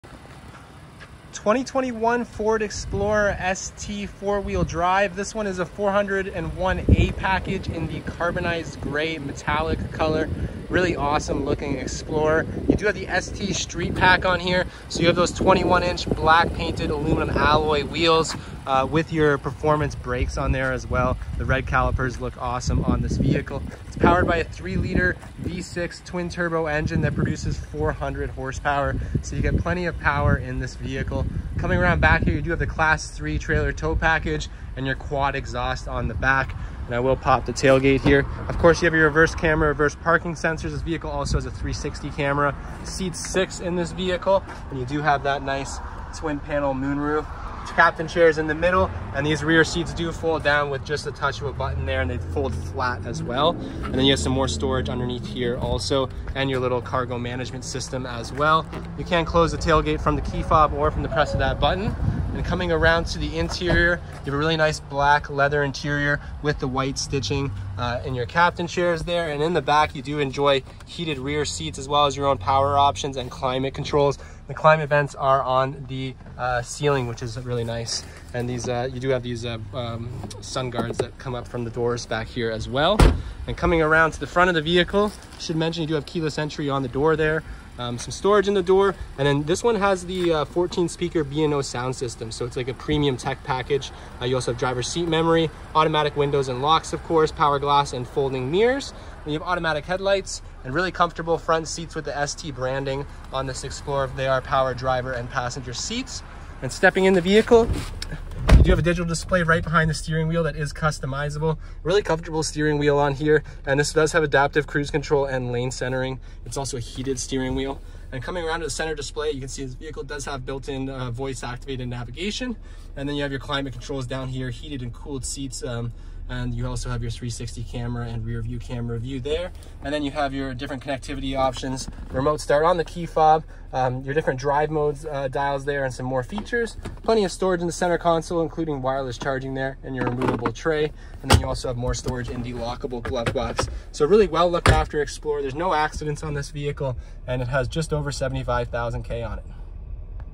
2021 Ford Explorer ST 4WD 401A w/ ST Street Pack in Carbonized Gray Metallic Walk-Around
ST 4WD 401A|Carbonized Grey Metallic|3.0L EcoBoost w/ 400hp|Twin Panel Moonroof|ST Street Pack w/ 21" Gloss Black Alum Wheels|Performance Brakes|Class III Trailer Tow Package|Premium Tech Package w/ 14-Speaker B&O Sound System|10.1" LCD Touchscreen|Ebony Leather Trimmed Interior|Heated and Cooled Power Front Seats|Heated 2nd Row Captain Chairs|Power Heated Mirrors w/ Memory|360 Camera|Front & Rear Parking Sensors|Adaptive Cruise ControlLane-Centering|Voice-Activated Navigation|Heated Steering Wheel|Remote Start|360 Camera|Universal Garage Door Opener|1st and 2nd Row Floor Liners|Cargo Area Management System|And Much More!